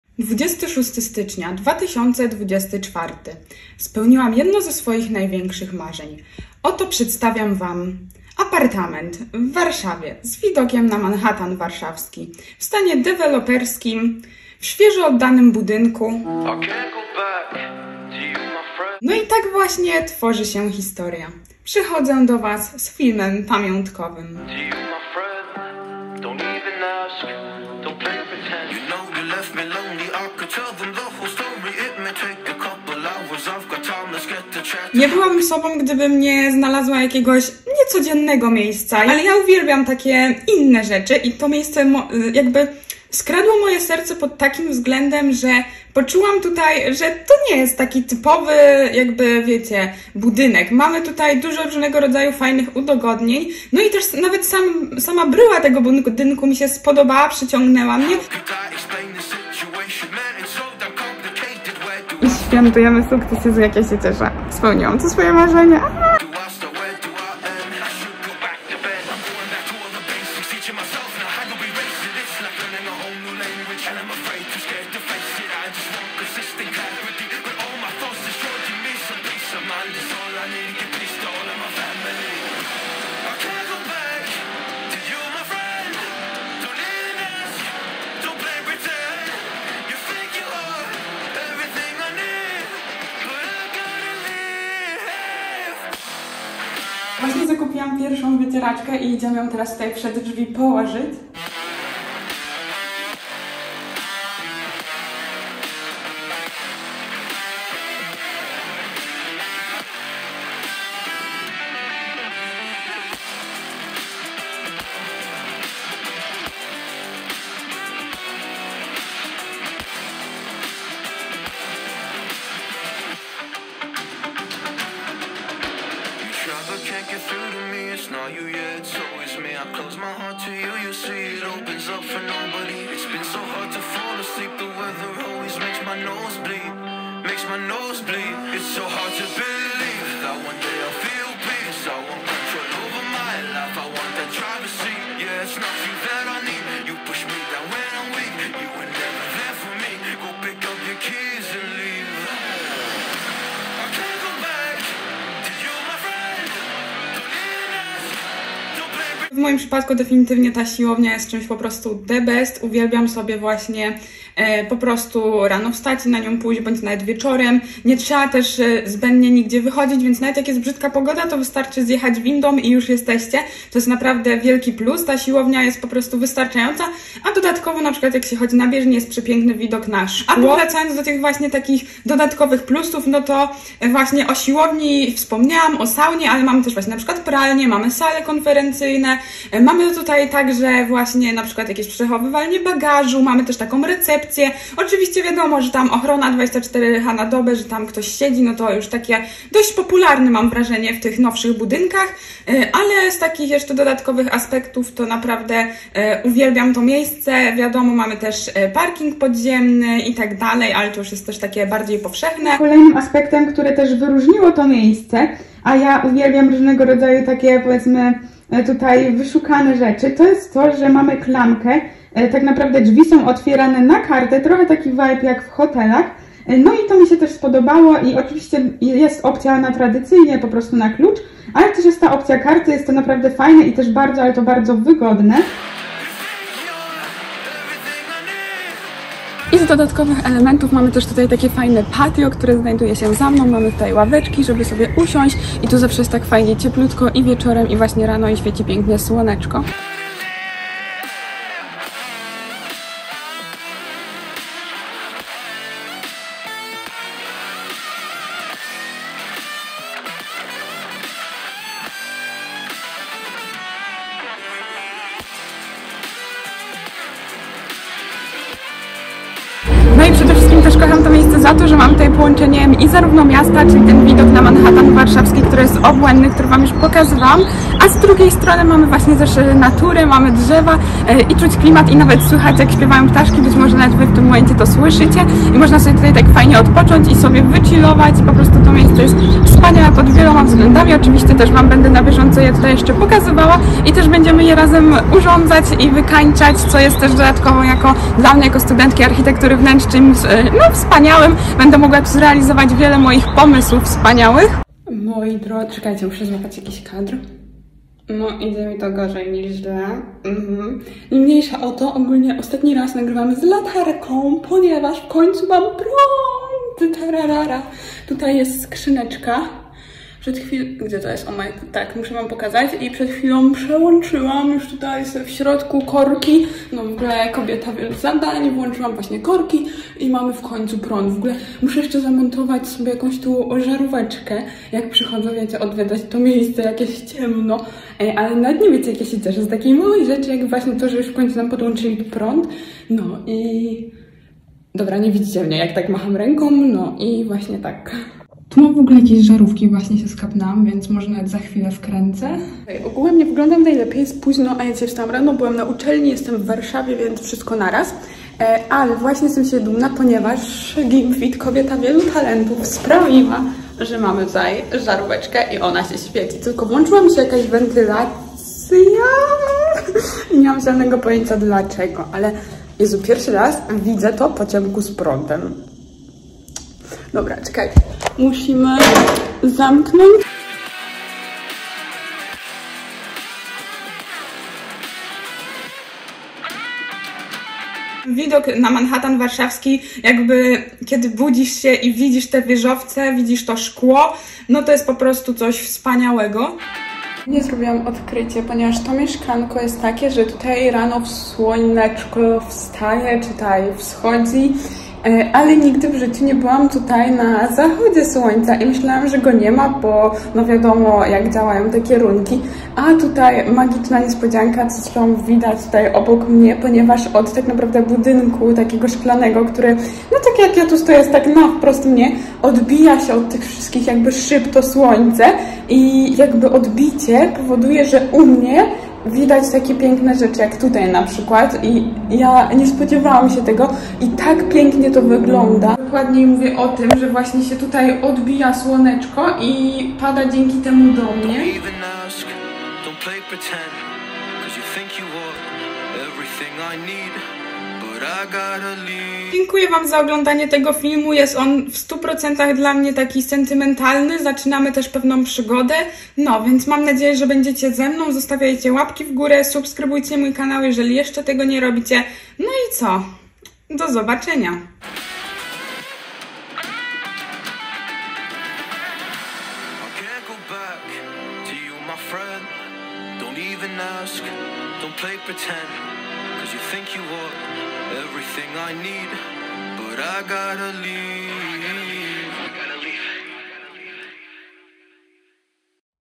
Apartament w Warszawie￼￼￼
Hej kochani tym razem przychodzę do Was z filmem pamiątkowym.✰ Życzę miłego oglądania.♡

Kilka słów ode mnie:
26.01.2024 r. spełniłam jedno ze swoich największych marzeń. Długo zwlekałam z tym, aby podzieleniem się z Wami tą informacją, ale oto przedstawiam Wam apartament w Warszawie z widokiem na Manhattan warszawski, w stanie deweloperskim.

Jako mała dziewczynka powtarzam sobie, że jak będę mieć 18 lat „będę dorosła” to będę miała apartament w Warszawie.

Udało się!

Jeśli na czymś Ci bardzo zależy i włożysz w to bardzo dużo pracy, to osiągniesz, nieosiągalne.

Bo tak właśnie, przez ostatnie ponad pół roku, ile razy i od ile osób usłyszałam, że jest to nieosiągalne- ciężko byłoby zliczyć.

Nie było łatwo, ale się udało, bo w momencie, kiedy każdy stracił jakąkolwiek nadzieję i wiarę, że mój pomysł jest do zrealizowania, jeżeli zostanie włożona ciężka praca, ja pomimo różnych emocji, które towarzyszyły mi przez ponad pół roku w związku z tym, nie zwątpiłam ani razu.

Nie było łatwo, ale wszystko, co złe zostawmy i skupmy się na tym, co przed nami, bo będzie się bardzo dużo działo.

Jest to pewnego rodzaju wyzwanie.

Jako studentka architektury wnętrz stwierdziłam, że podzielę się tą informacją, ponieważ chcę razem z Wami przeżywać proces wykańczania tego apartamentu.

Gwarantuję Wam, że pomysł mam idealny.

 ➭ Pamiętaj aby udostępnił moje filmy swoim koleżankom i kolegom
 ➭ Ten film nie powstał w celu chwalenia się

✰tik tok ➭ gimfitnew

-as always i love you guys♡